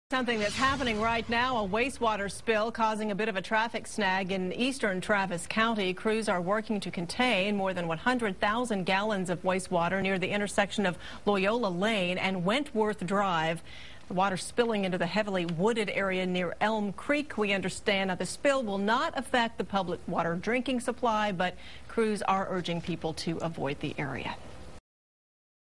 Wastewater spill into Elm Creek
More than 100,000 gallons entered into nearby Elm Creek in a heavily wooded area in far eastern Travis County late Monday.